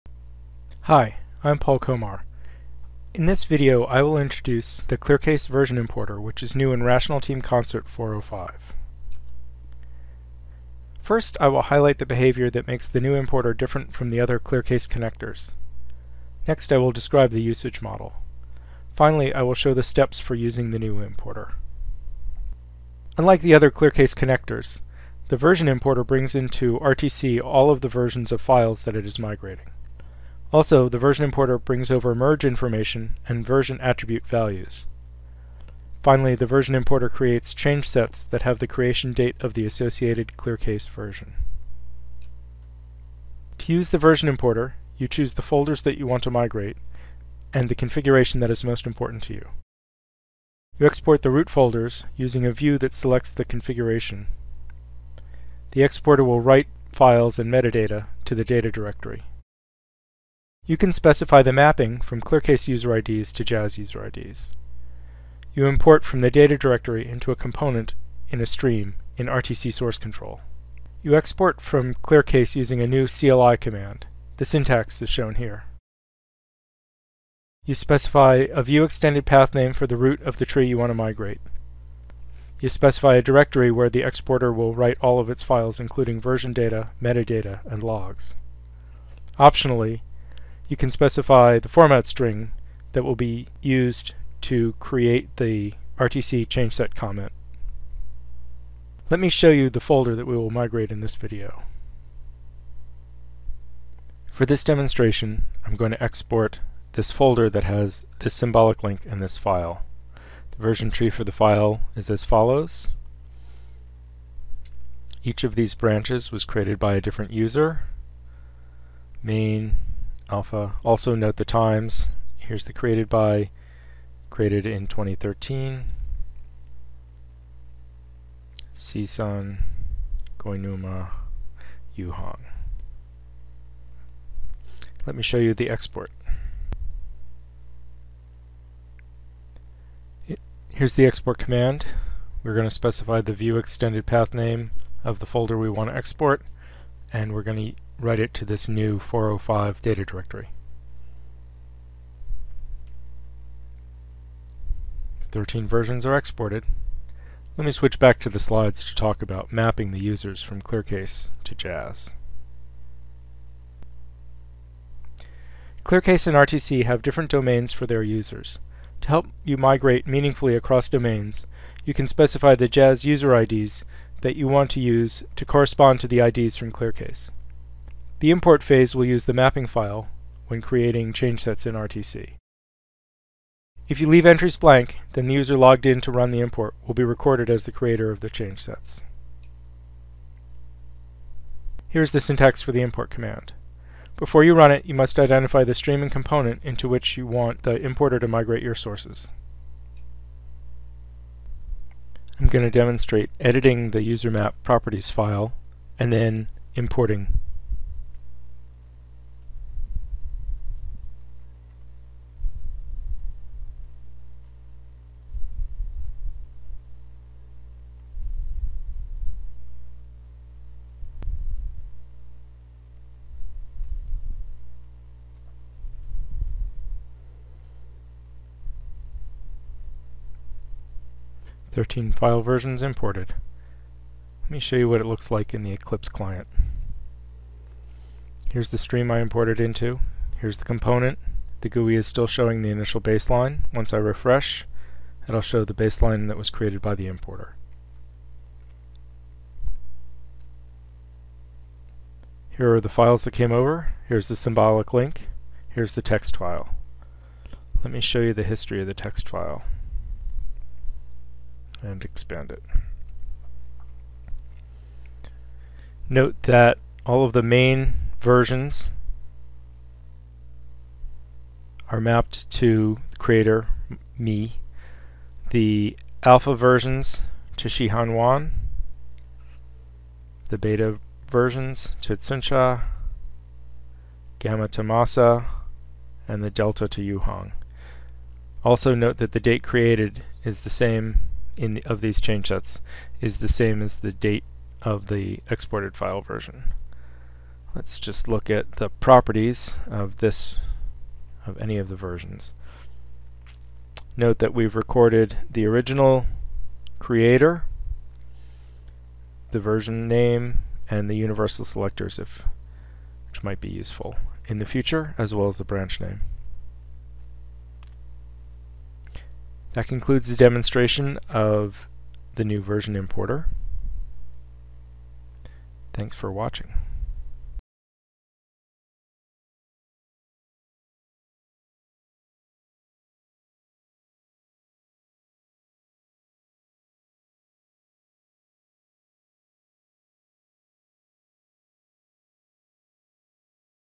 ClearCase Importer features for Rational Team Concert 4.0.5
Features for the importer used to migrate ClearCase to Rational Team Concert source control.